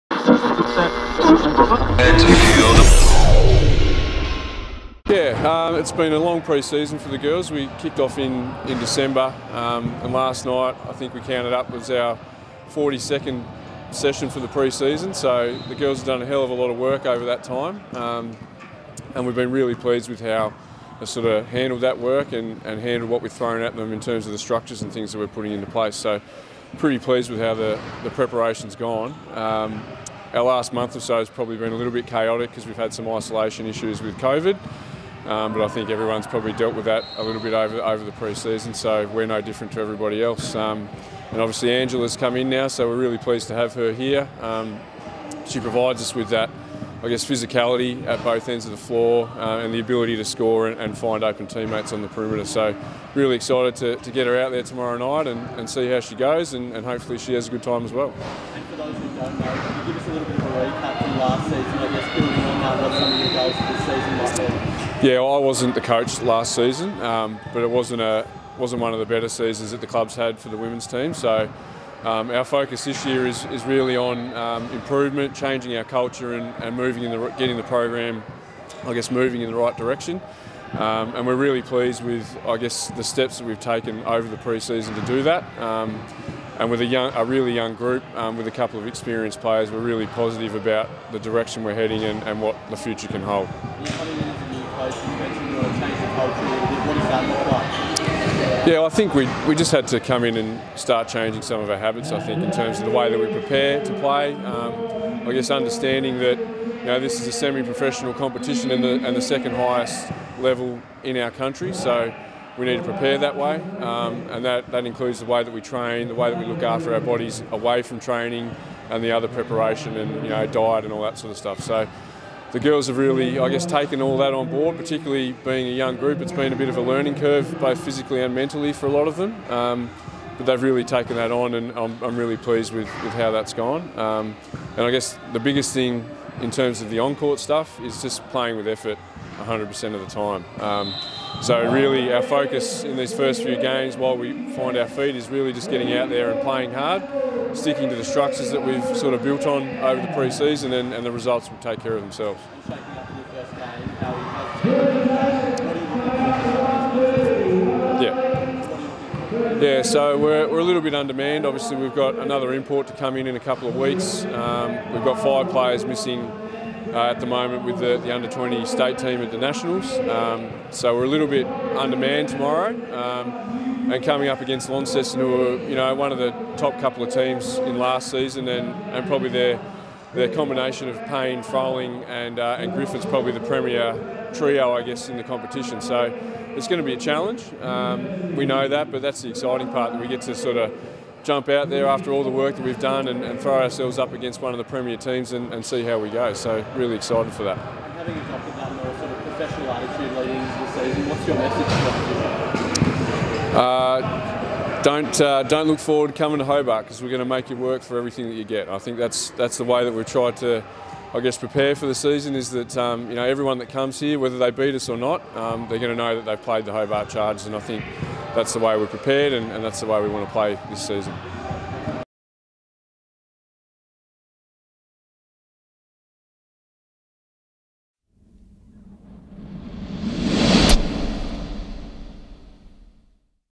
Hobart Chargers Press Conference 22/4/22 Dwain Davie
Hear from Women's Head Coach Dwain Davie as he speaks about pre season and looking ahead to Saturday night's game against arch rivals the Launceston Tornadoes